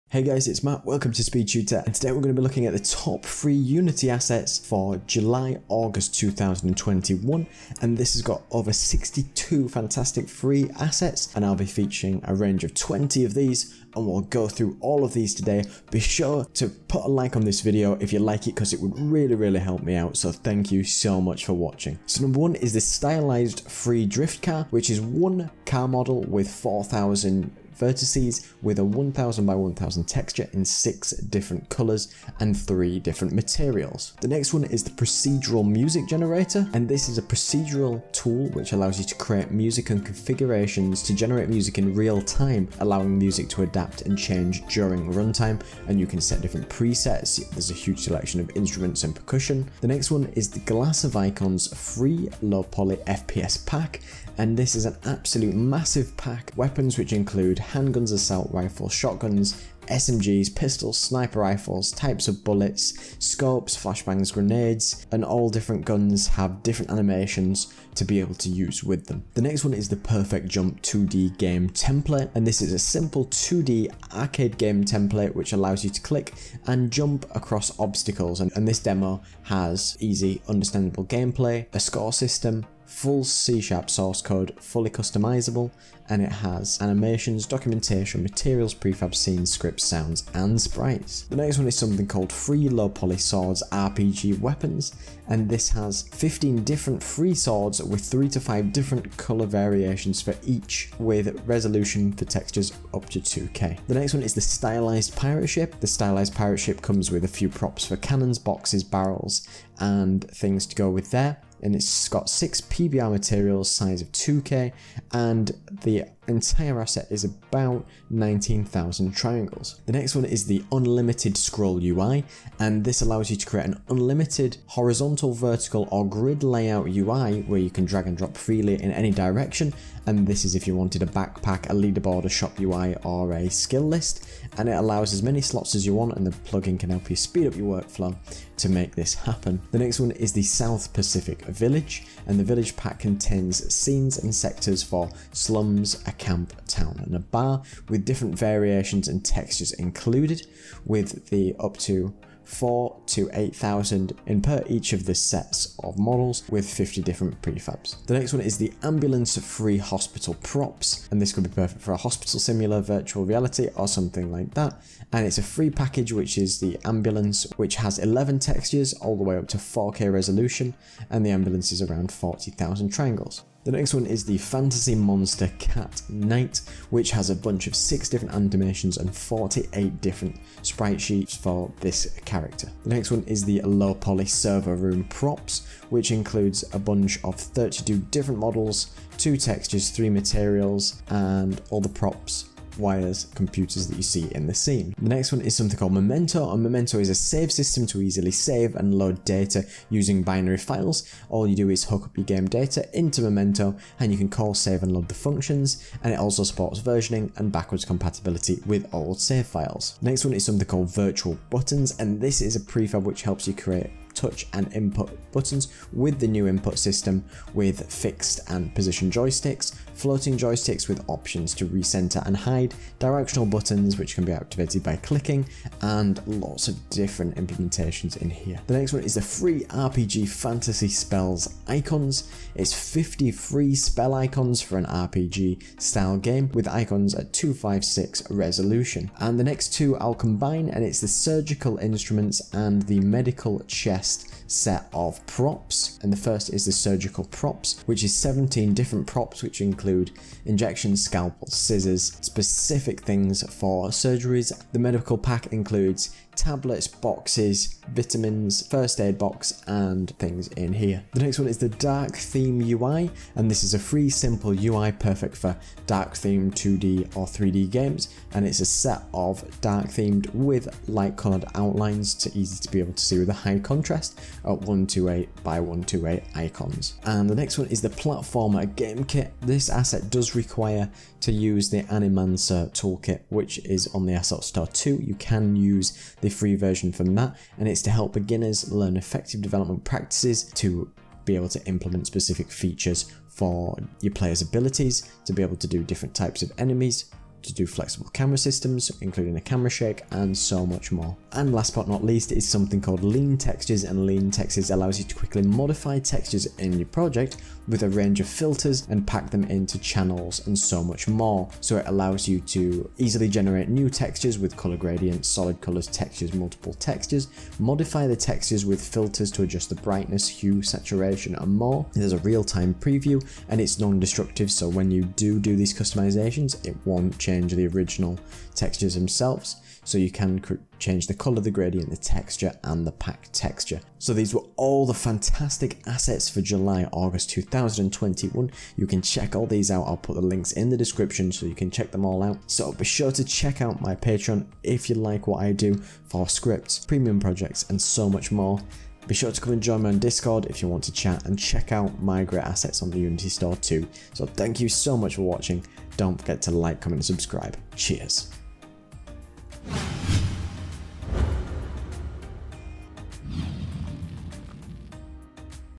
FREE Unity Assets - August 2021 | Unity Asset Store
A selection of the best free assets for Unity, for July / August. This includes 62 Free models, Programming Assets, Materials, Textures, Particles, VFX and more!

1). Stylized: Free Drift Car
4). Perfect Jump - 2D Game Template
5). Free Low Poly Swords - RPG Weapons
9). Ambulance - Free Hospital Prop
10). Fantasy Monster: Cat Knight
11). LowPoly Server Room Props
14). Free RPG Fantasy Spell Icons